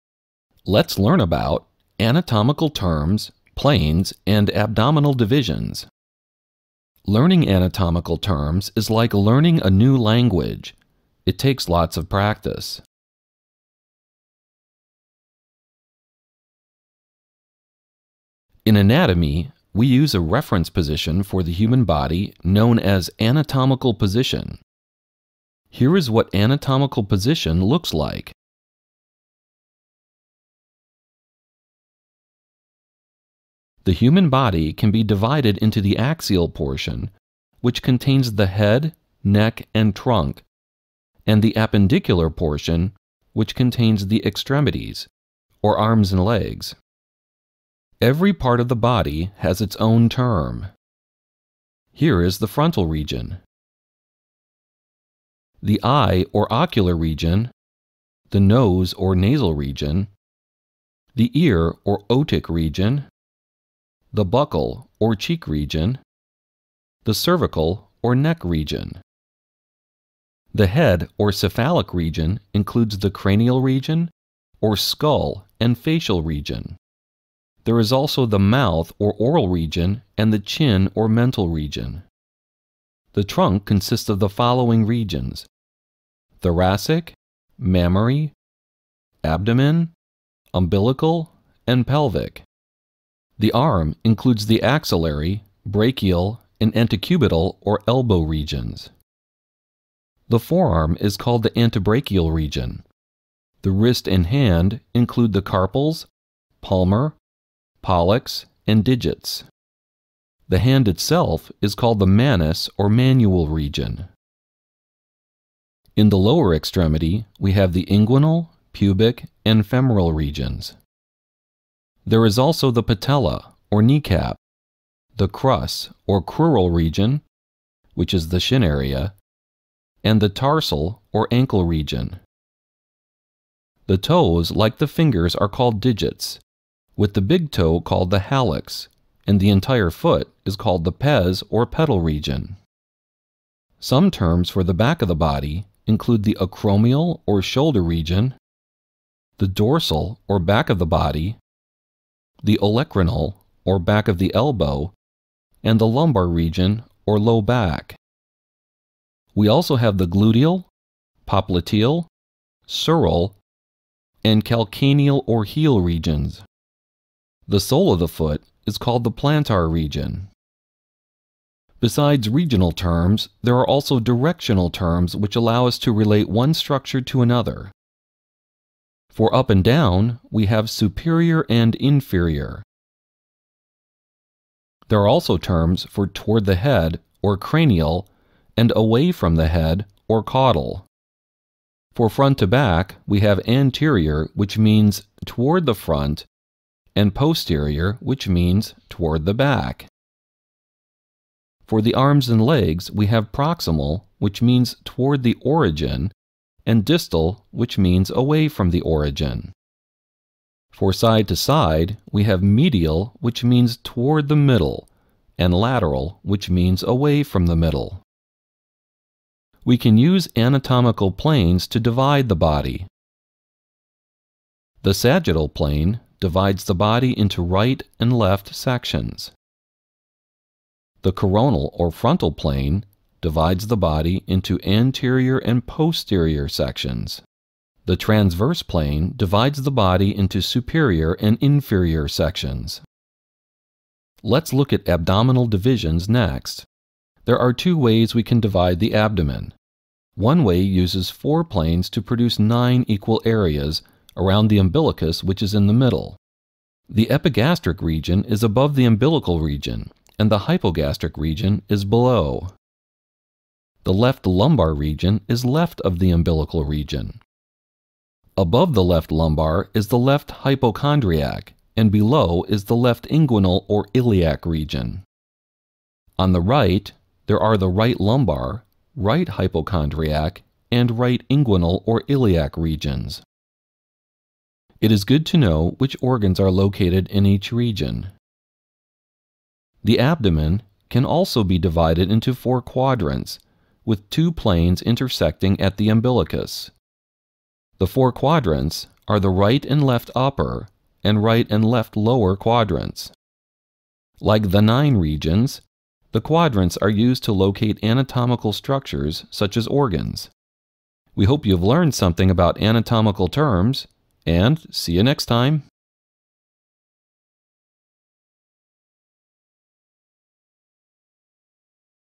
Anatomy and Physiology: Anatomical Terms V2.0
Overview of anatomical terms for Anatomy and Physiology (no music version).